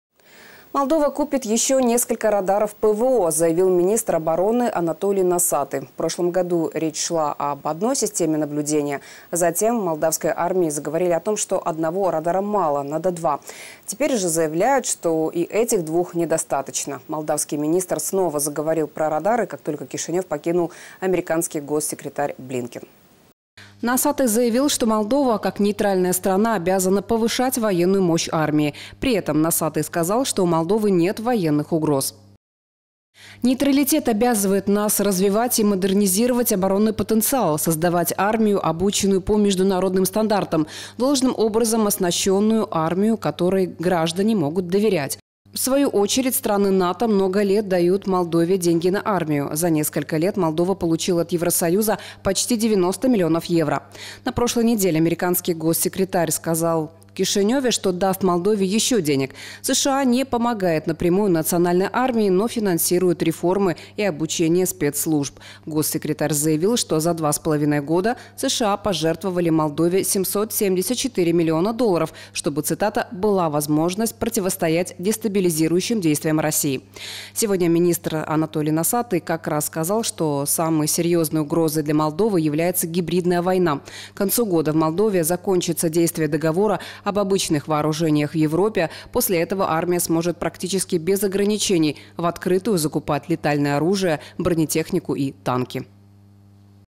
Молдова купит ещё несколько радаров
Молдова купит ещё несколько радаров ПВО, заявил министр обороны Анатолий Носатый. В прошлом году речь шла об одной системе наблюдения, затем в молдавской армии заговорили о том, что одного радара мало, надо два. Теперь же заявляют, что и этих двух недостаточно. Молдавский министр снова заговорил про радары, как только Кишинёв покинул американский госсекретарь Блинкен.